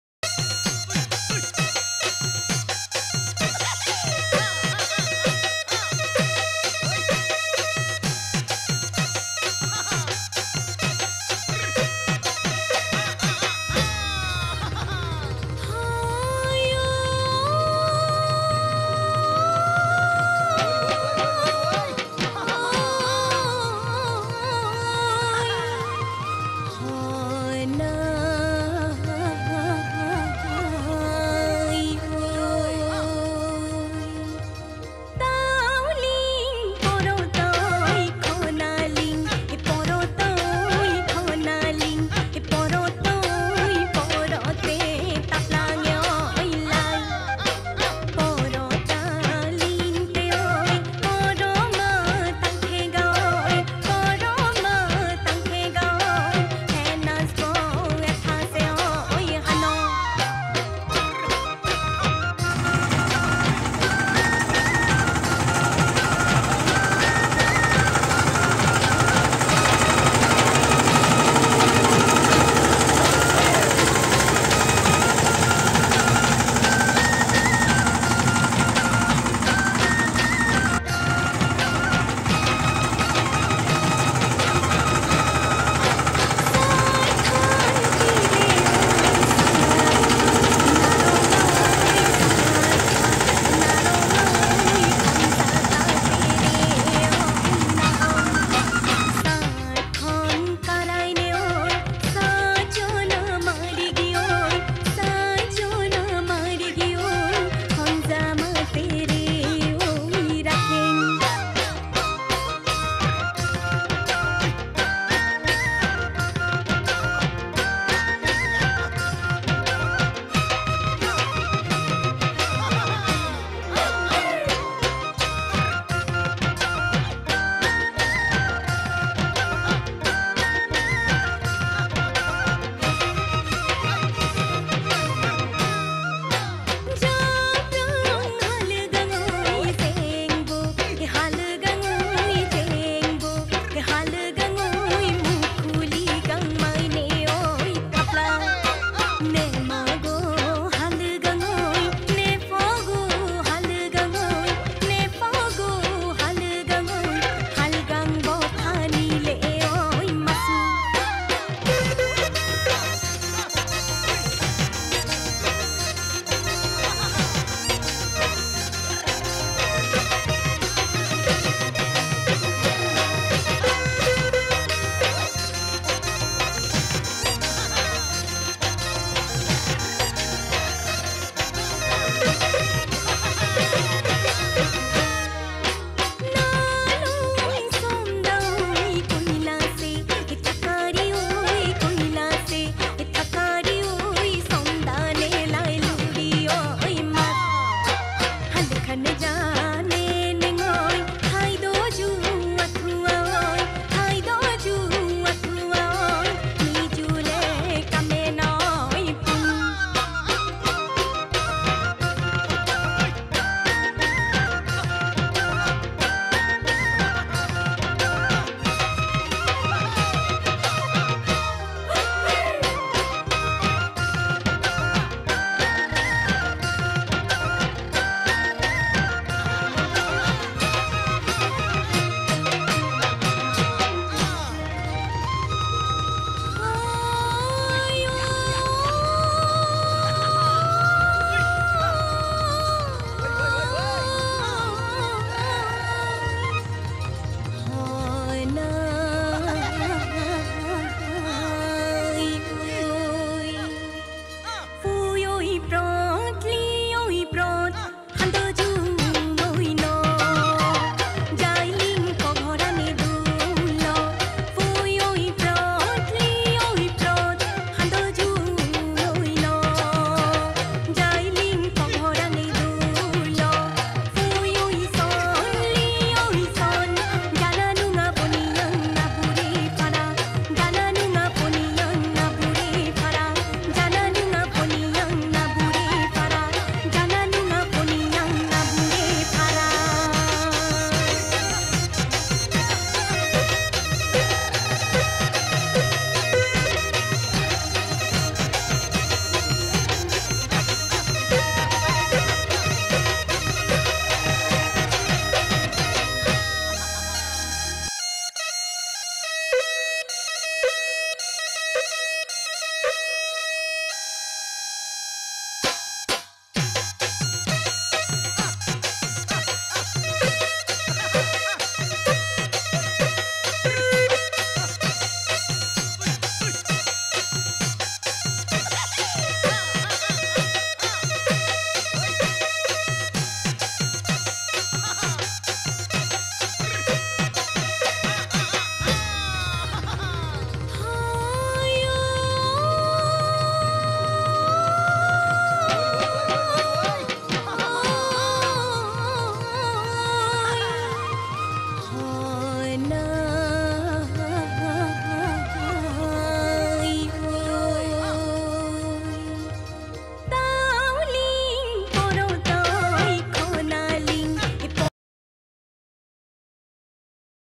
# Assam Paddy farming / ploughing tractor / Paddy sapling picking planting ধান খেতি কৰাৰ পদ্ধতি
# Assam Paddy farming / ploughing tractor / Paddy sapling / picking and planting  ধান খেতি কৰাৰ পদ্ধতি